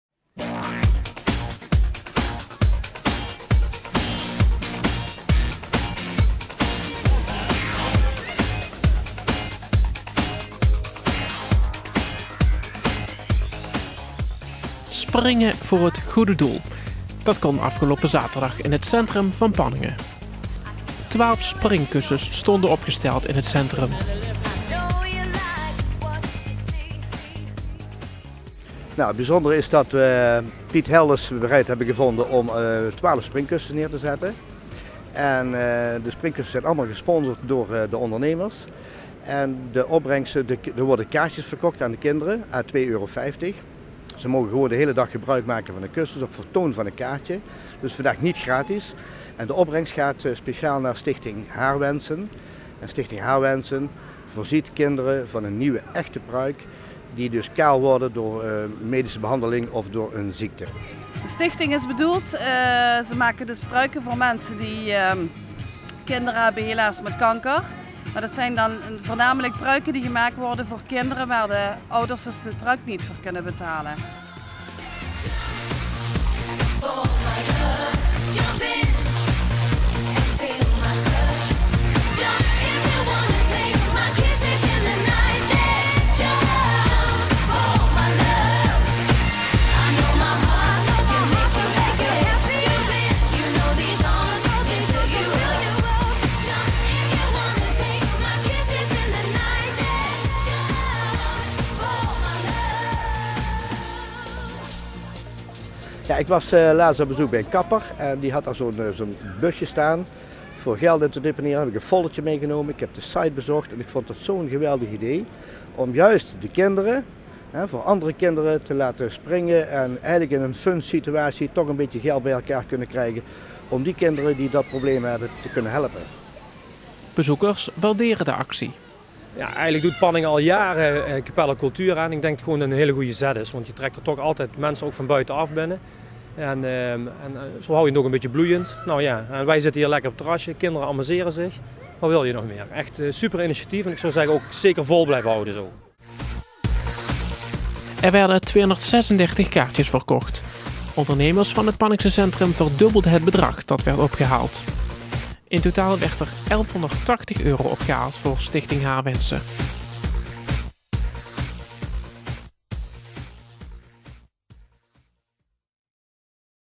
Kinderen springen voor Stichting Haarwensen
In Panningen hebben 236 kinderen meegedaan aan Jump voor Stichting Haarwensen. Er werd geld opgehaald voor deze stichting.